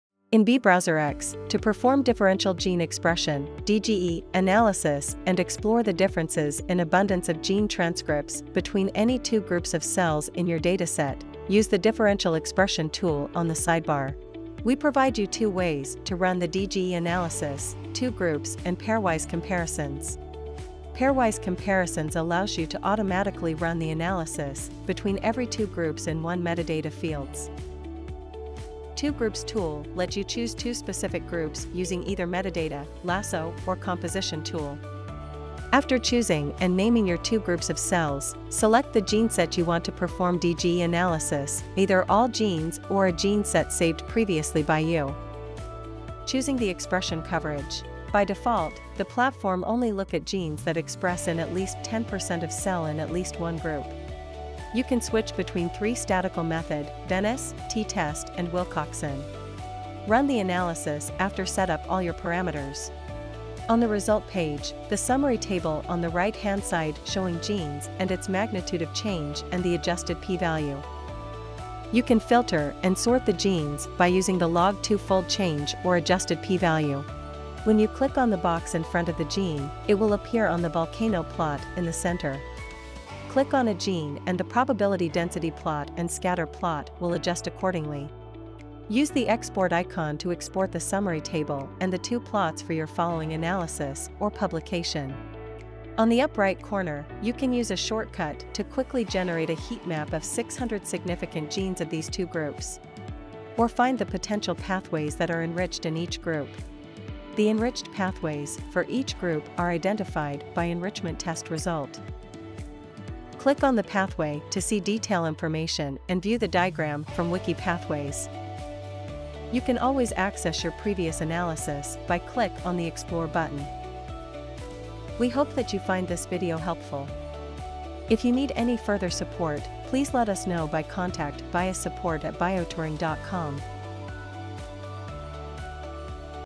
BBrowserX: DGE analysis
DGE is an essential step in any single-cell pipeline. In this tutorial, we'll guide you through the simple process using BBrowserX to perform DGE on any dataset.